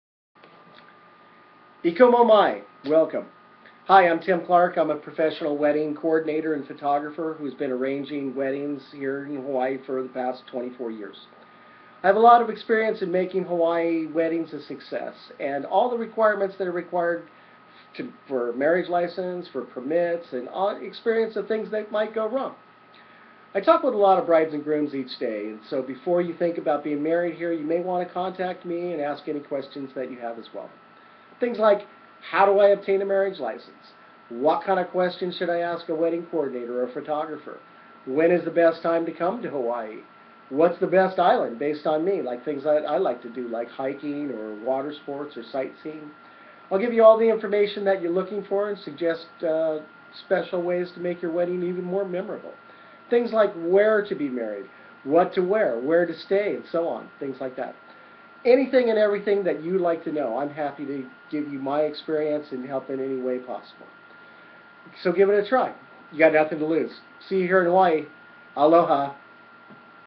Tim Clark President Hawaiian Island Weddings, Inc.
Do you have questions about being married in Hawaii? I can help answer all your questions.